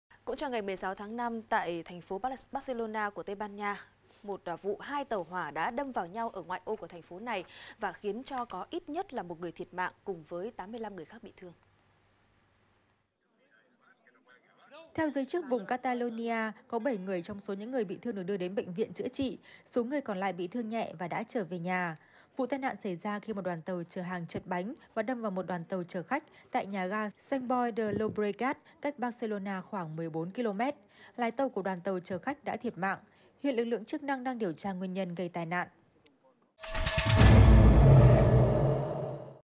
TÂY BAN NHA: TAI NẠN TÀU HỎA, 1 NGƯỜI THIỆT MẠNG, 85 NGƯỜI BỊ THƯƠNG | VTC9
VTC9 | TÂY BAN NHA: TAI NẠN TÀU HỎA, 1 NGƯỜI THIỆT MẠNG, 85 NGƯỜI BỊ THƯƠNG